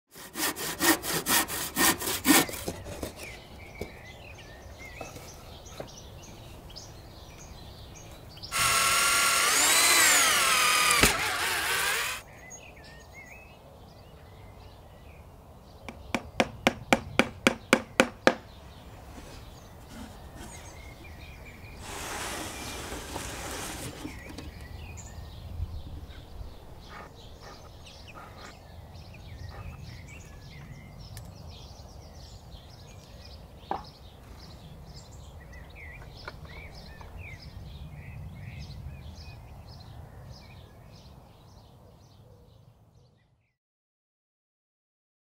Create a sparrow street & help give nature a home
Watch this short video on how to encourage these charming birds to call your house a home.